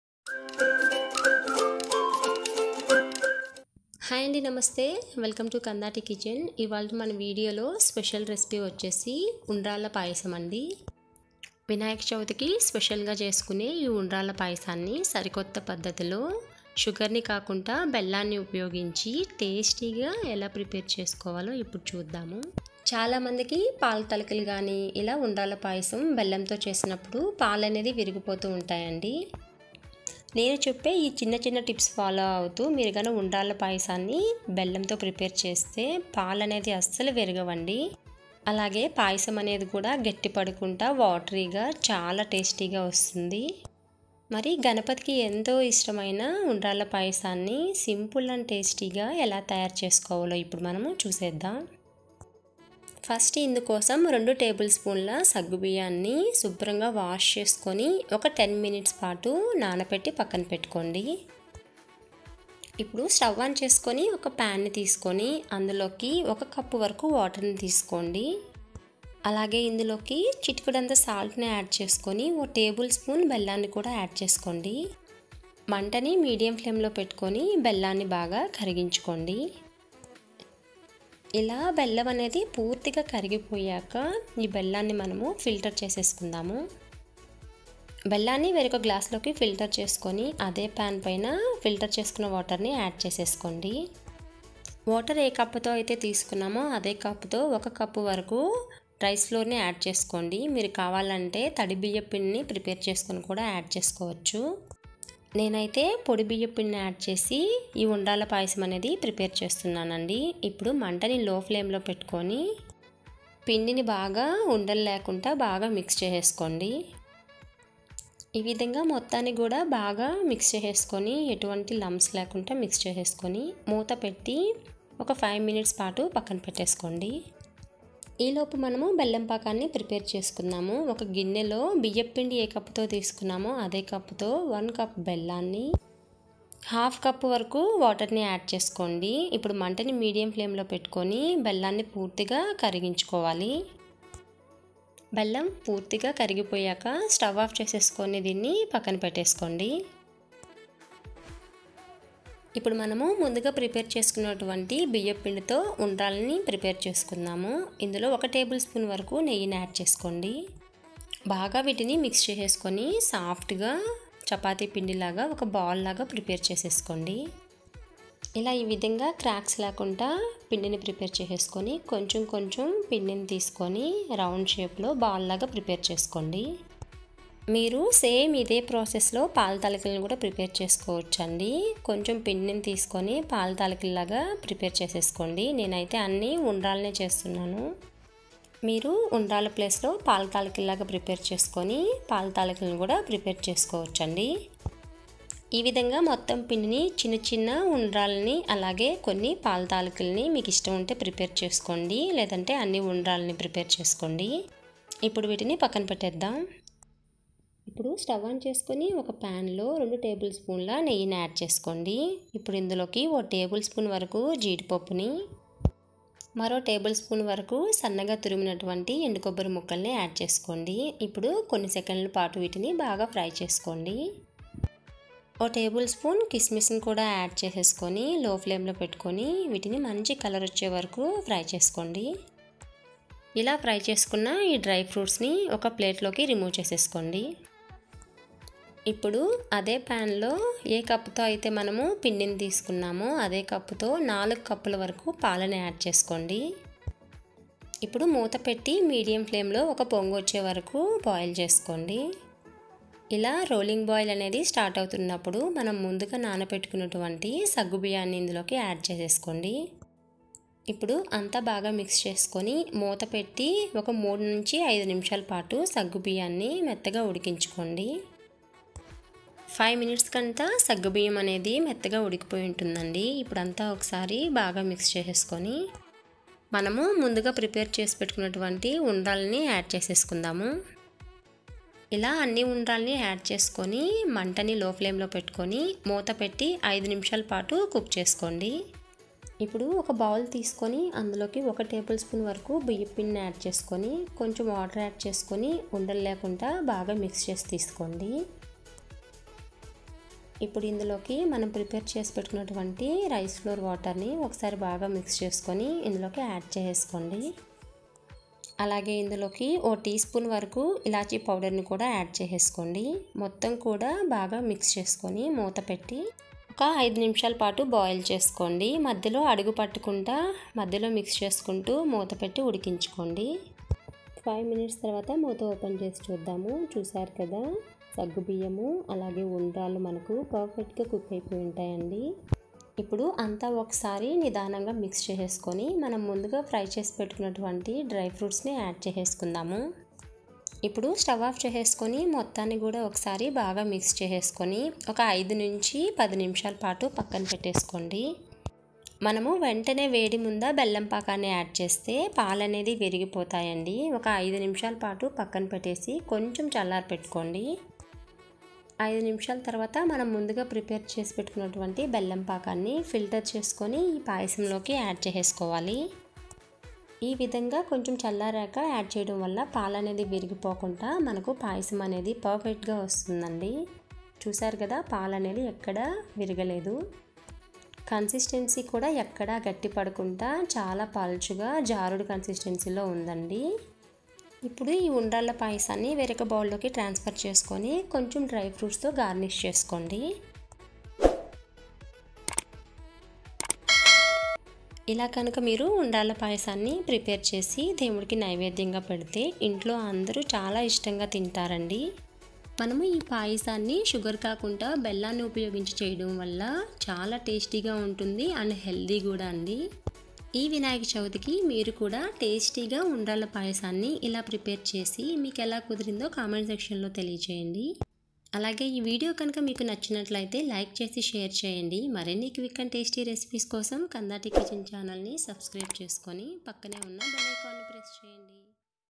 ఉండ్రాళ్ళ పాయసం పాలు విరగకుండా మరింత రుచిగా రావాలంటే ఇలా చేయండి👍👌| Undralla Payasam|Joggery Payasam
More Prasadham Recipe videos

purnam burelu Recipe 👉